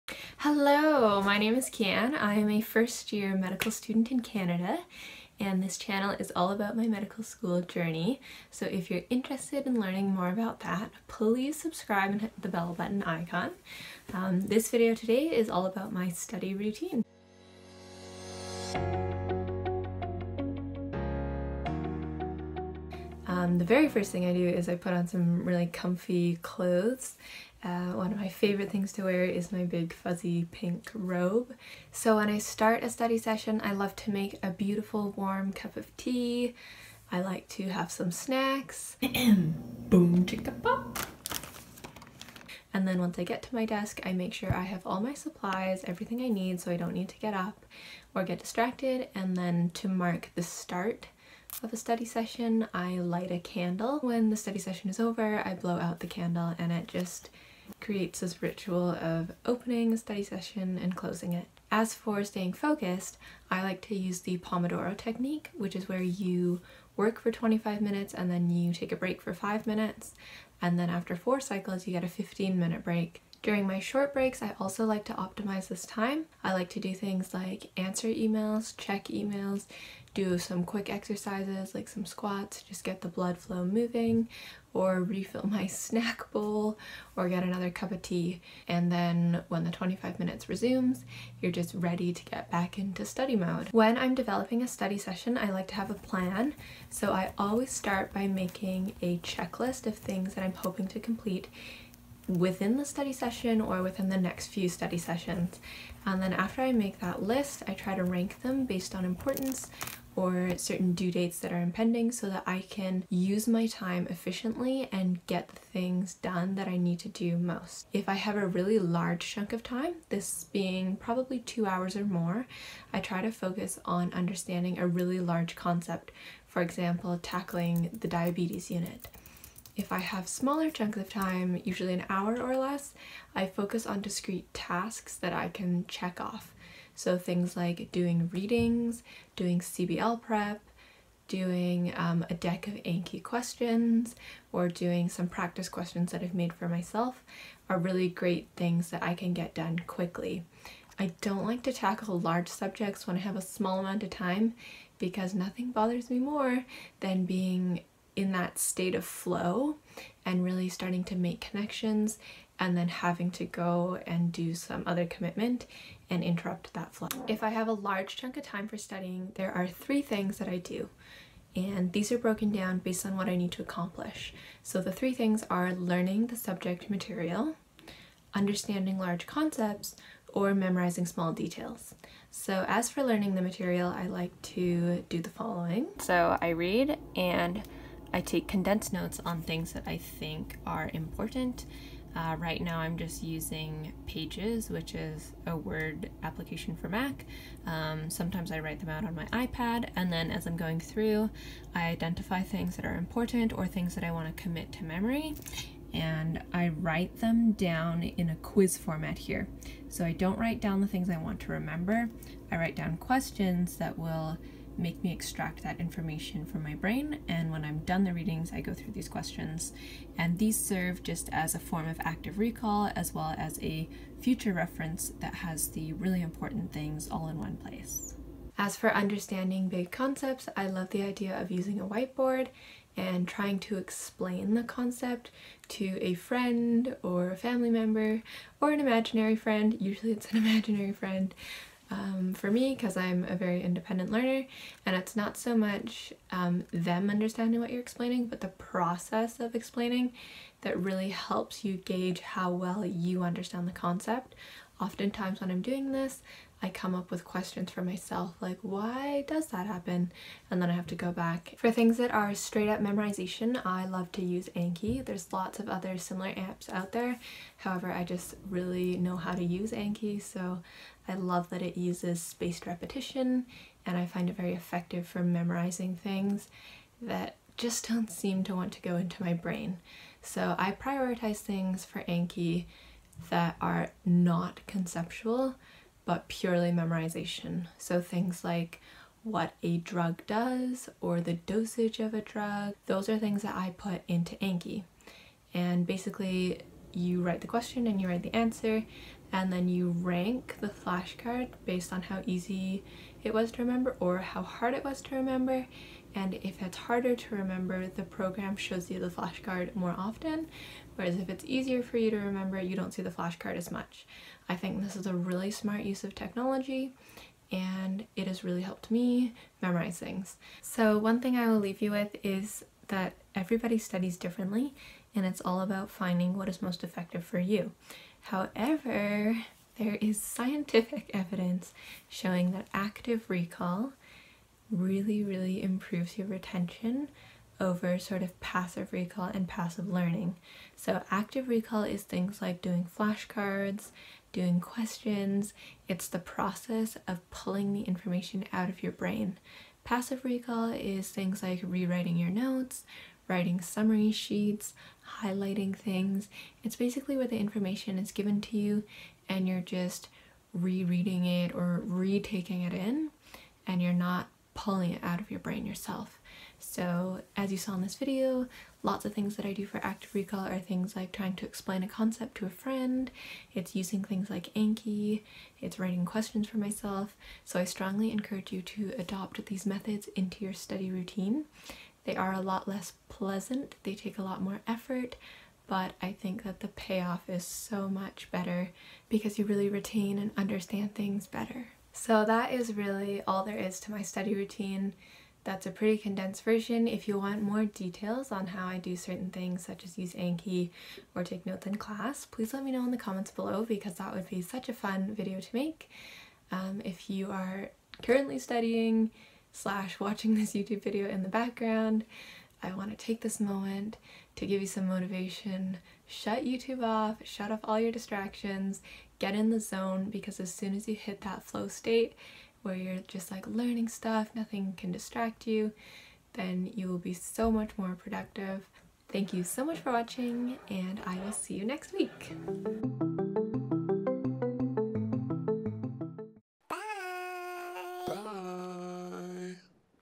Study Routine in Medical School
So this is how the magic (studying) happens, hope you enjoy this study routine video

I'm Kian, I have a quirky sense of humour and I'm a first year med student